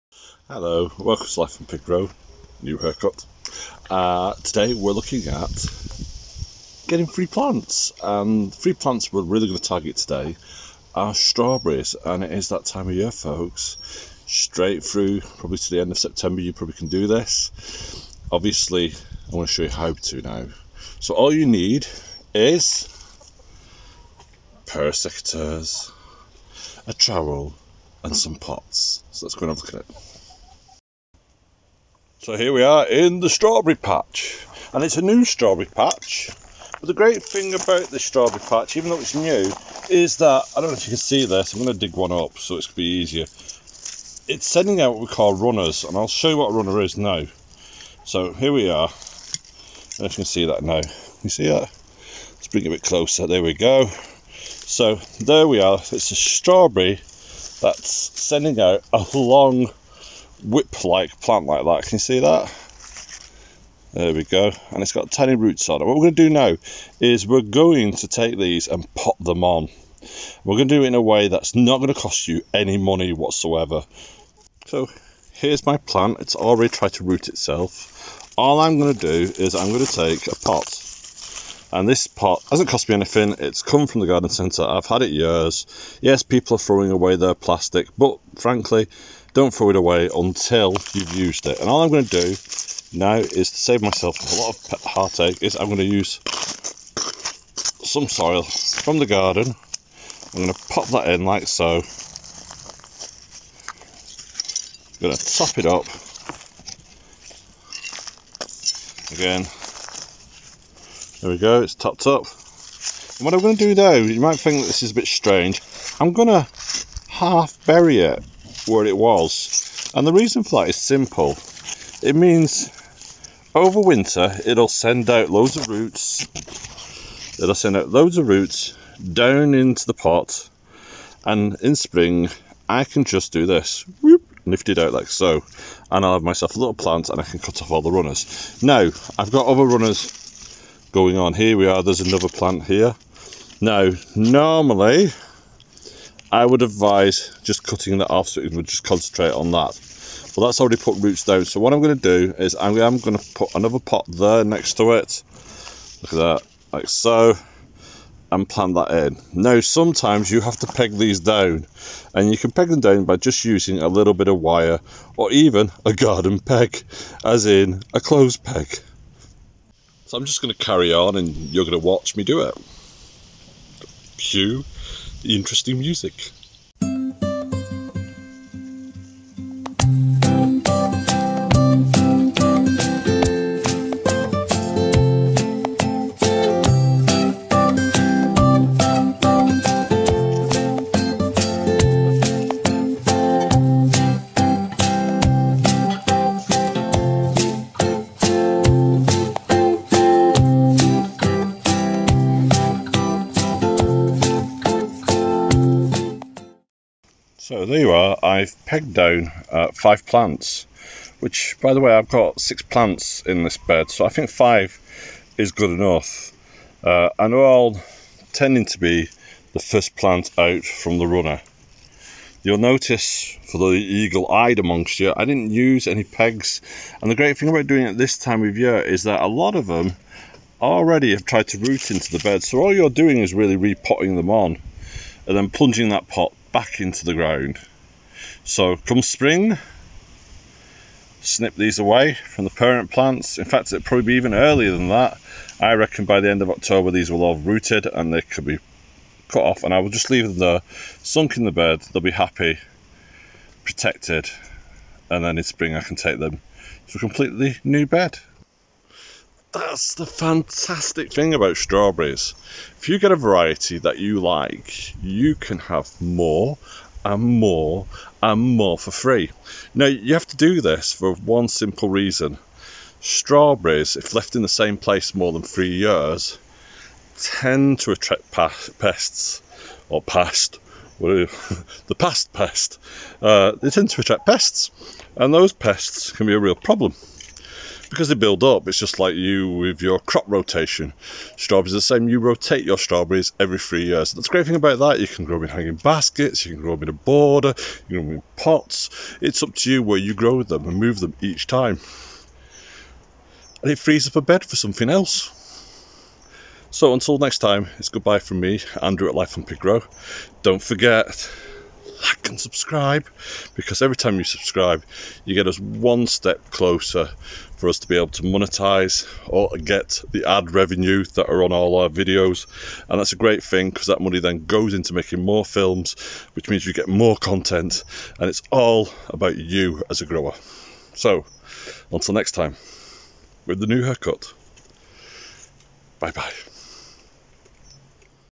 Get free strawberry plants
You have to the end of the month to get those free strawberry plants and learn why you should move your strawberries every few years.